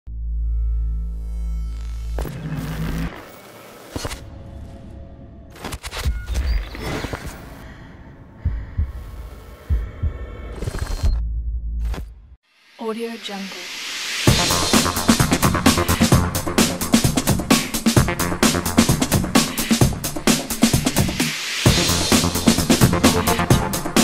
Plexus Titles 3 (Colorful Outburst) | After Effects Template
Special Thank You ❤ to theNullObject!

Recommended Section:
0:01 Animated Typeface
1:06 Download 'Social Media Opener'
1:06 Download 'Fast Trailer Teaser'
1:06 Music
1:14.200 Download 'Subscribe project'
Attention! This project requires Optical Flares, Plexus 2, Trapcode Particular. There are 1 media and 16 text placeholders.  The price is $22.